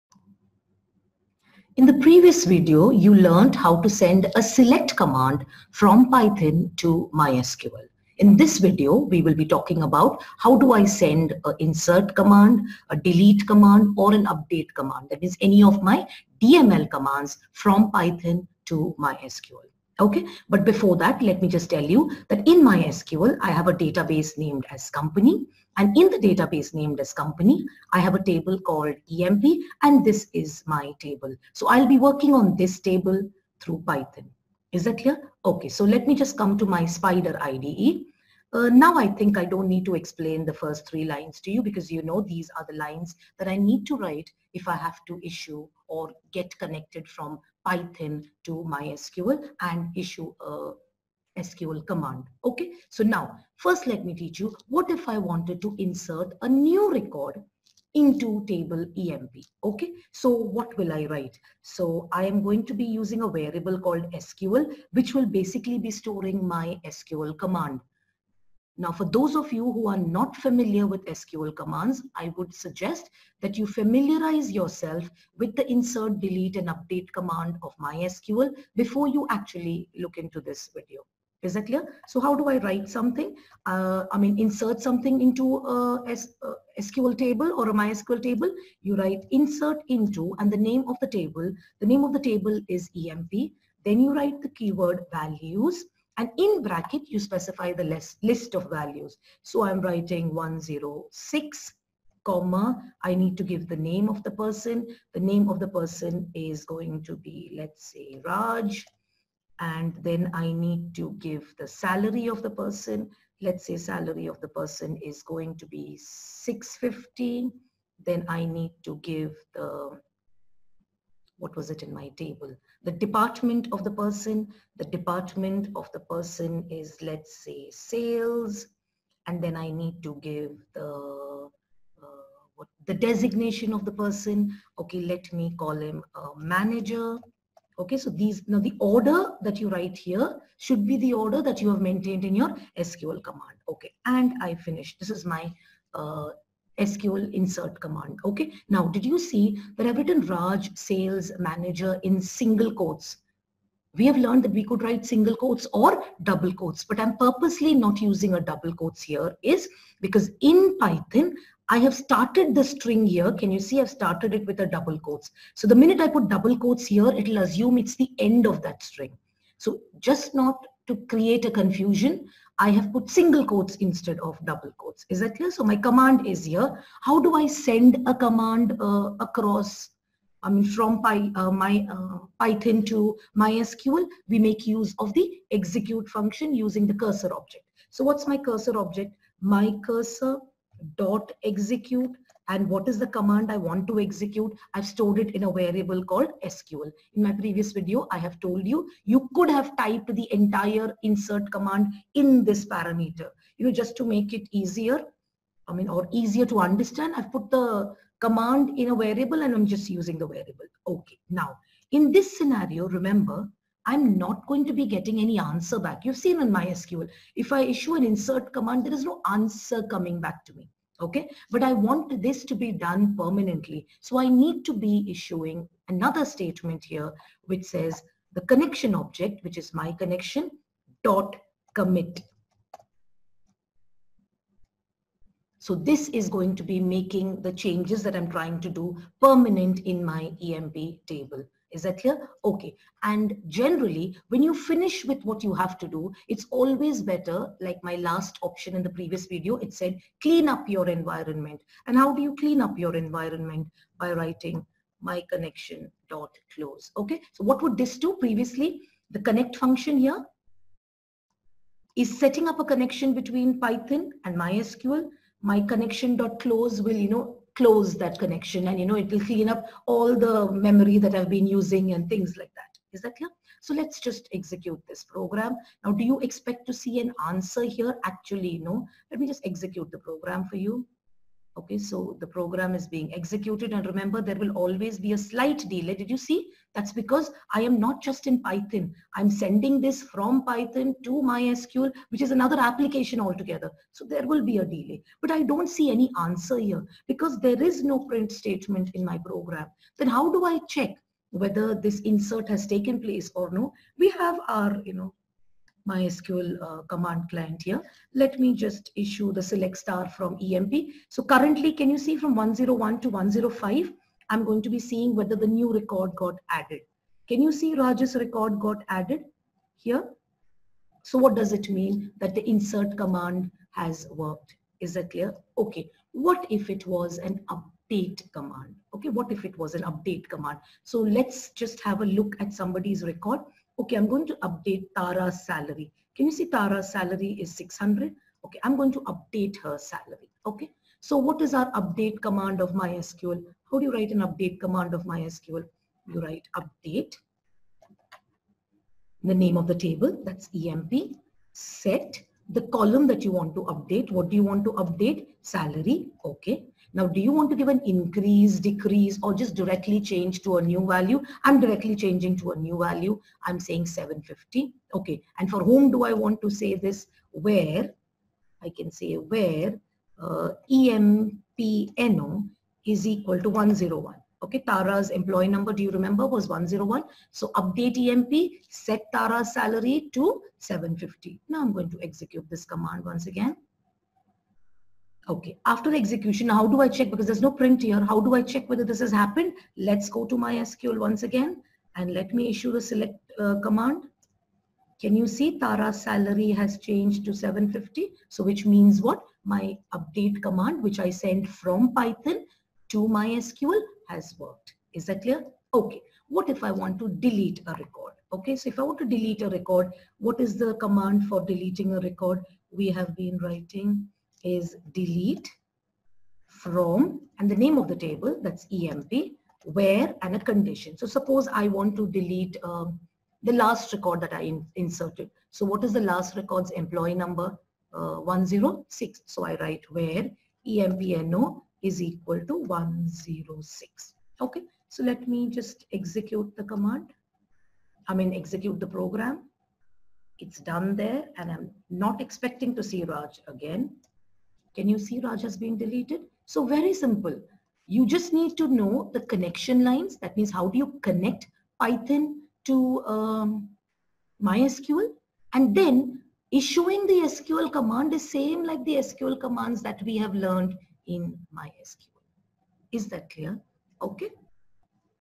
Interface Python with MySQL - Part 2 (Class XII)
In this video we will learn:
* how to send SELECT, DELETE and UPDATE command to MySQL from Python.